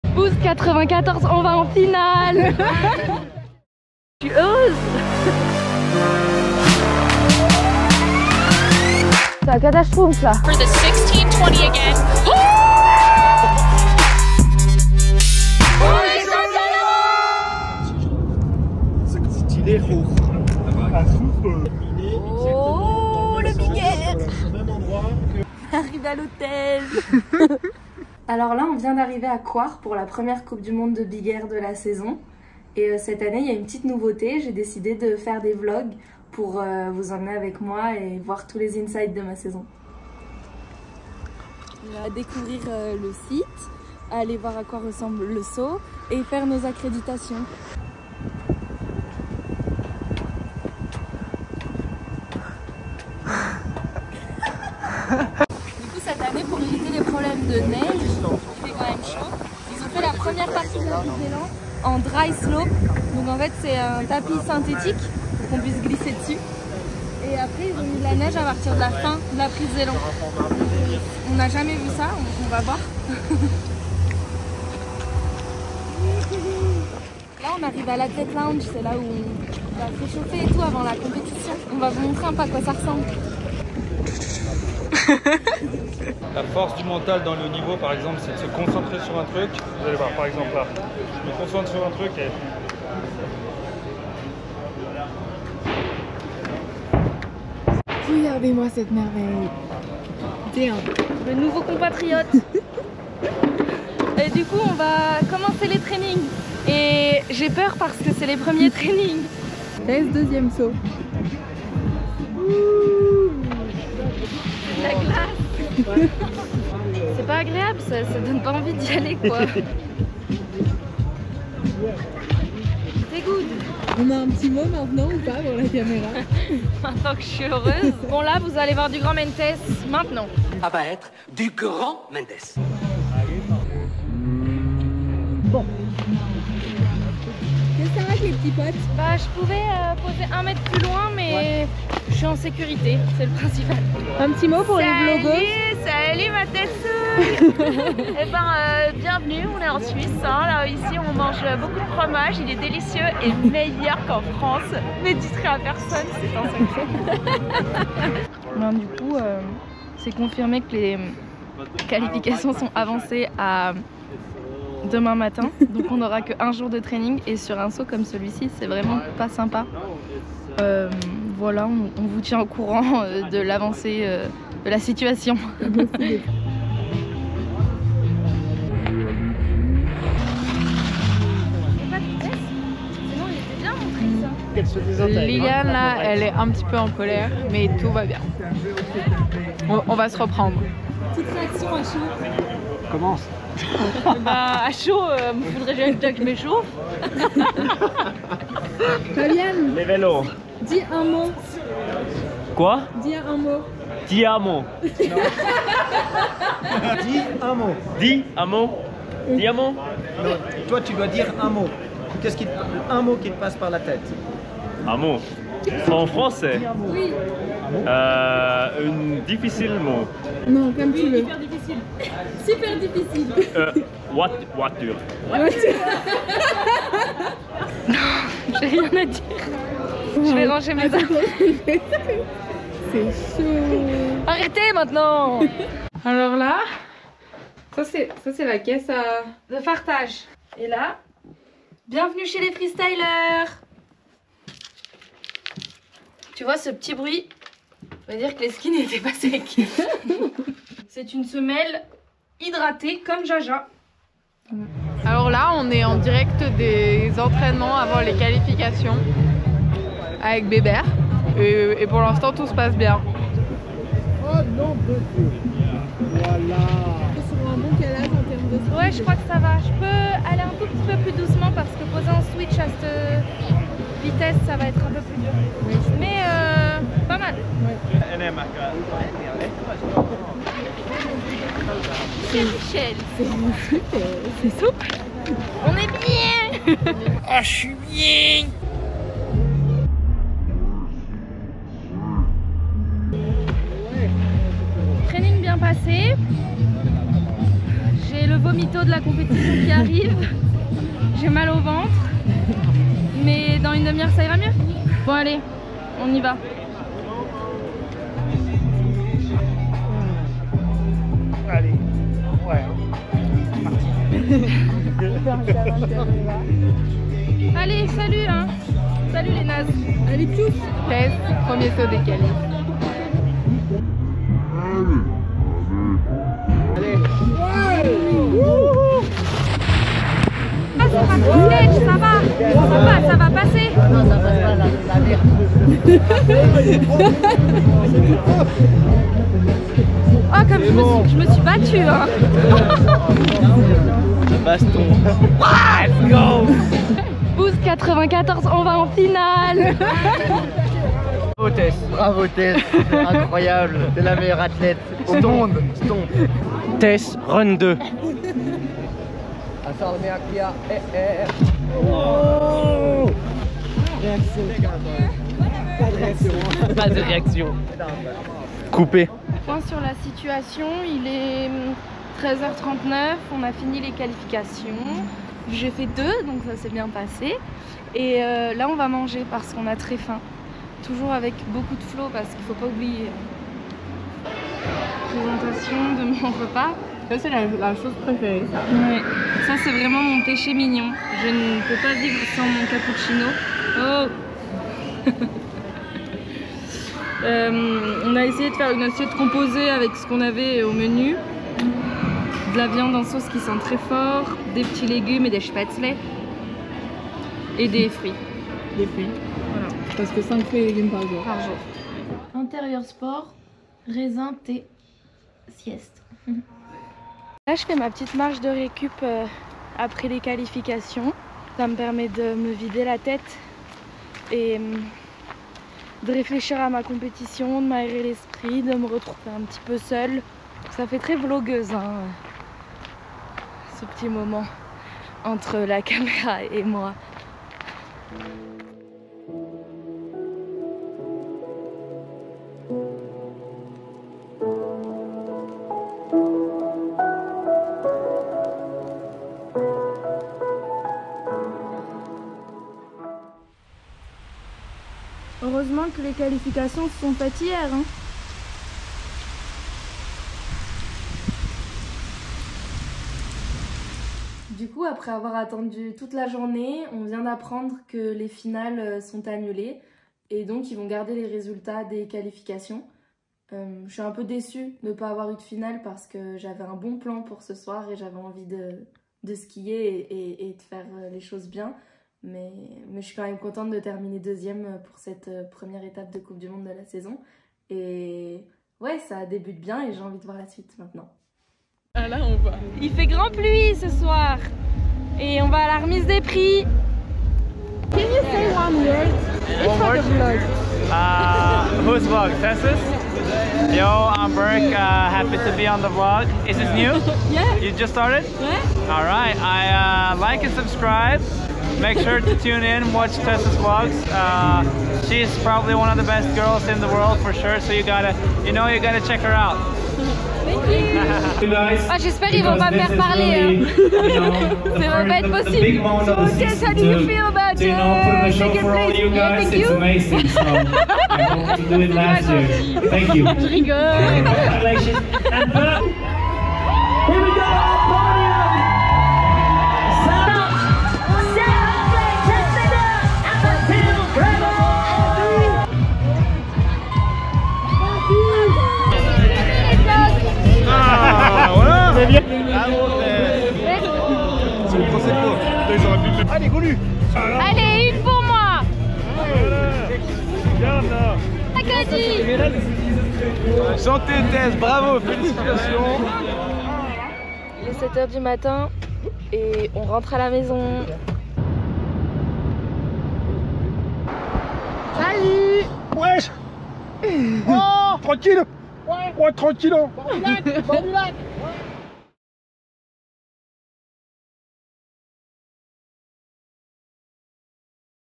Première coupe du monde de la saison ! - Vlog Tess Ledeux
Pour ce premier vlog, je vous emmène avec moi sur la première coupe du monde de big air de la saison en Suisse sur laquelle j'ai eu quelques surprises...
N'hésitez pas à aimer, commenter et partager si le contenu vous a plu !